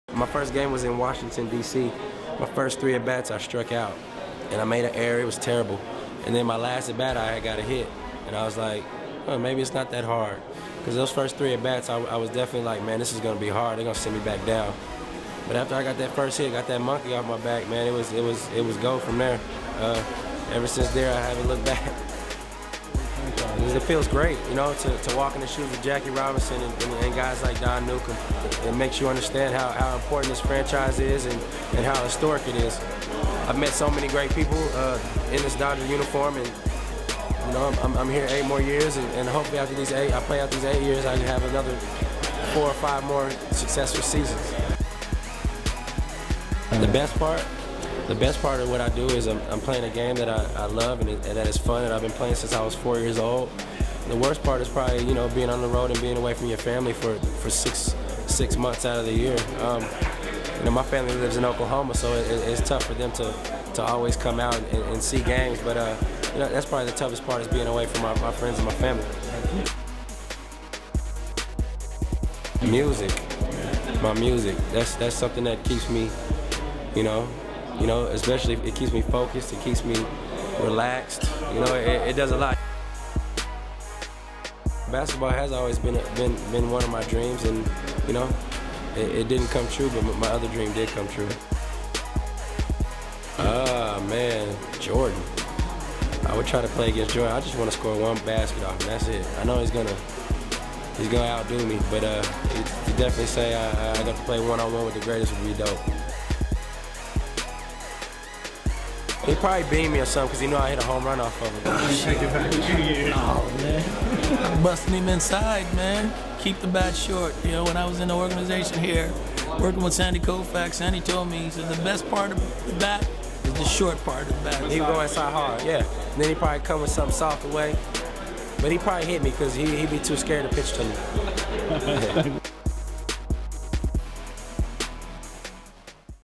Matt Kemp Goes Deep with DUB
Fresh from  signing the largest deal in Dodger's history, All-Star center fielder, and potential 2011 MLB MVP, takes a minute to talk to DUB.
Check back with DUB Magazine for more from Matt Kemp.